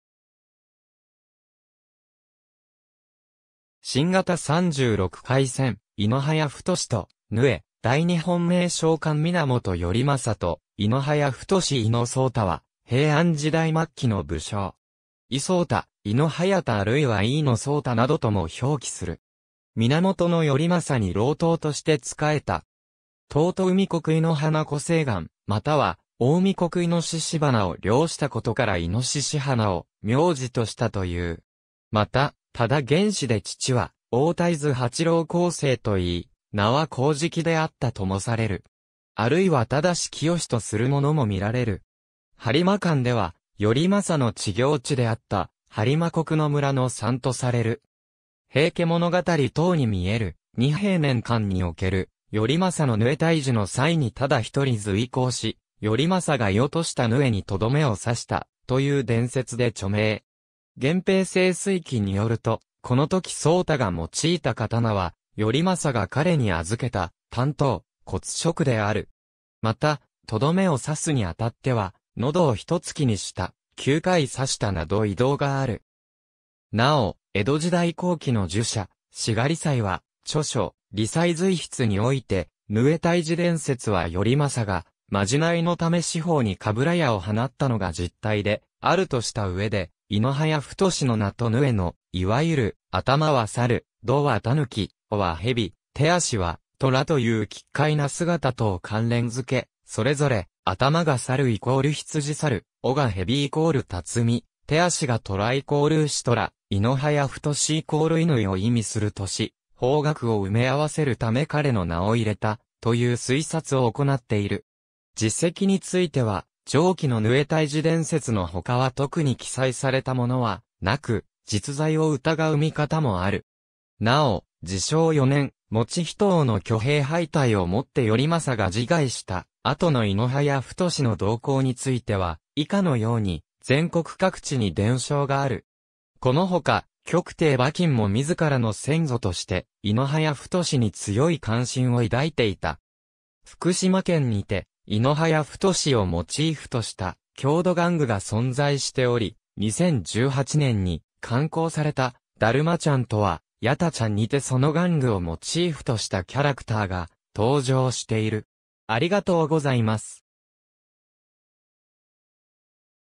猪早太
新形三十六怪撰：猪早太と鵺 大日本名将鑑　源頼政と猪早太 猪 早太（い の はやた、生没年不詳）は、平安時代末期の武将。
井 早太、猪 隼太あるいは猪野 早太などとも表記する。
源頼政に郎党として仕えた。
遠江国猪鼻湖西岸（現在の静岡県浜松市北区三ヶ日町）、または近江国猪鼻（現在の滋賀県甲賀市土山町猪鼻）を領したことから猪鼻を苗字としたという。
また、多田源氏で父は太田伊豆八郎広政（廣政）といい、名は高直であったともされる。
（後年の浮世絵などでは名を広直（廣直）あるいは忠澄とするものも見られる。
）『播磨鑑』では、頼政の知行地であった播磨国野村の産とされる。
『平家物語』等にみえる、仁平年間（1151-54、近衛天皇治世）における頼政の鵺（鵼）退治の際にただ一人随行し、頼政が射落とした鵺にとどめを刺した、という伝説で著名。
『源平盛衰記』によると、このとき早太が用いた刀は、頼政が彼に預けた短刀「骨食」である。
また、とどめを刺すに当たっては「喉を一突きにした」、「九回刺した」など異同がある。
なお、江戸時代後期の儒者・志賀理斎（忍）は著書『理斎随筆』において、鵺退治伝説は頼政がまじないのため四方に鏑矢を放ったのが実態である（いわゆる奉射神事）とした上で、猪早太の名と鵺のいわゆる“頭は猿、胴は狸、尾は蛇、手足は虎（異同もある）”という奇怪な姿とを関連付け、それぞれ「頭が猿＝未申（南西）」「尾が蛇＝辰巳（南東）」「手足が虎＝丑寅（北東）」「猪早太＝戌亥（北西）」を意味するとし、方角を埋め合わせるため彼の名を入れた、という推察を行っている。
事跡については上記の鵺退治伝説のほかは特に記載されたものはなく、実在を疑う見方もある。
なお、治承4年（1180年）以仁王の挙兵敗退をもって頼政が自害した後の猪早太の動向については、以下のように全国各地に伝承がある。
このほか、曲亭馬琴も自らの先祖として猪早太に強い関心を抱いていた。
福島県にて猪早太をモチーフとした郷土玩具が存在しており、2018年に刊行された『だるまちゃんとはやたちゃん』にてその玩具をモチーフとしたキャラクターが登場している。